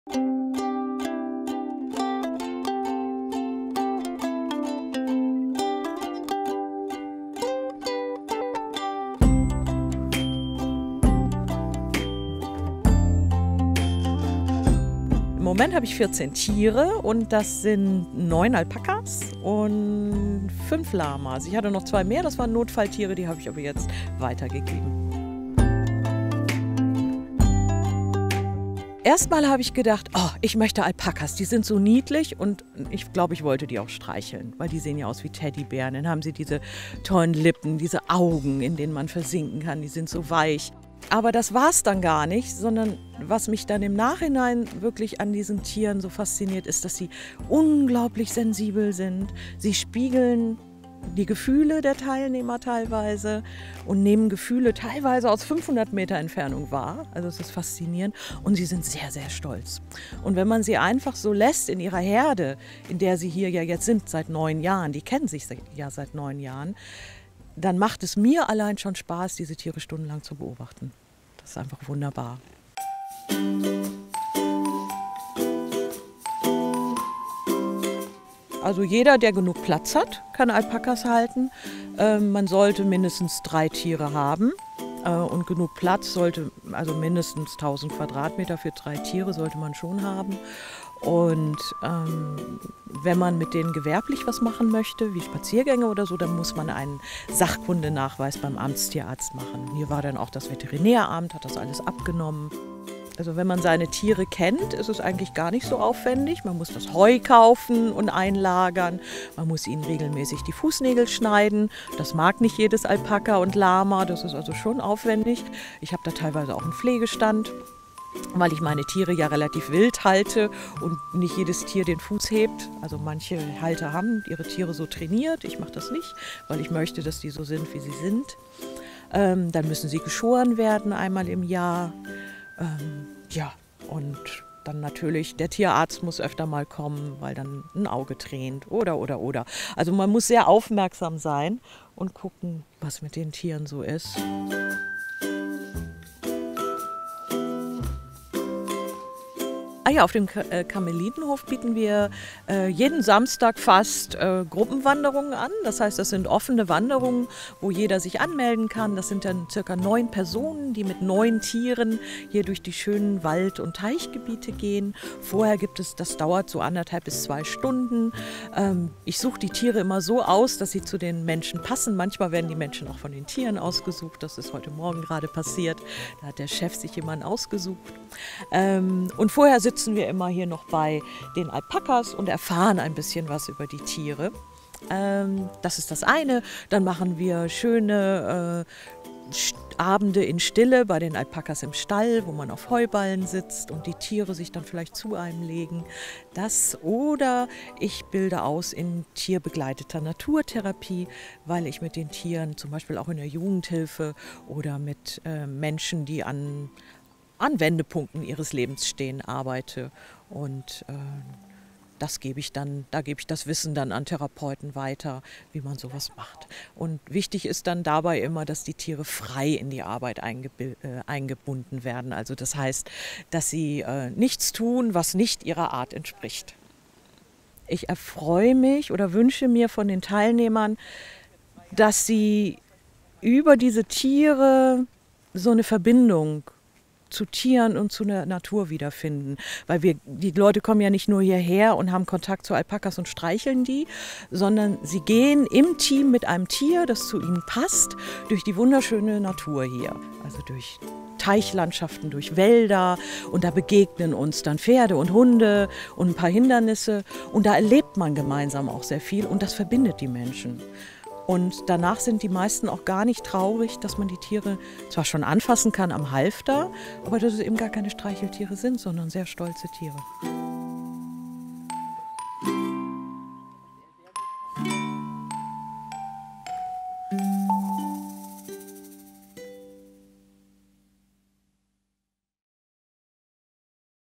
Alpakas – Mehr als nur flauschig
Michaela Molzahn liegt mit ihrem Camelidenhof im Trend. Denn hier in Varel sind Alpakas und Lamas zuhause. Die sind aber nicht zum Schmusen da, vielmehr sollen sie bei Spaziergängen den Besuchern die Natur und Ruhe näher bringen. Wir haben uns auf dem Hof umgesehen und mit ihr über die Besonderheiten und Faszination der Tiere gesprochen.